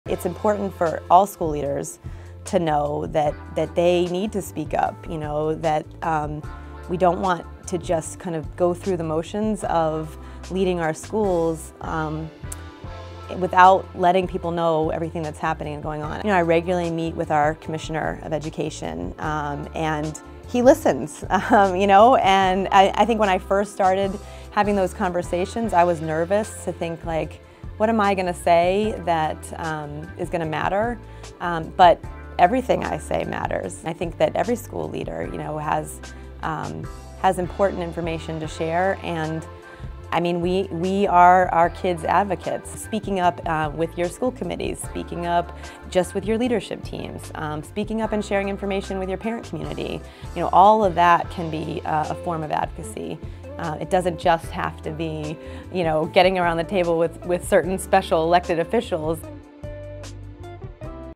fromthefield | Liz Garden
When PrincipalsAdvocate, they inspire change. In an @NAESP FromTheField video, principal @PrincipalGarden discusses how the edleader voice drives positive change in education. "It's important for all school leaders to know that they need to speak up."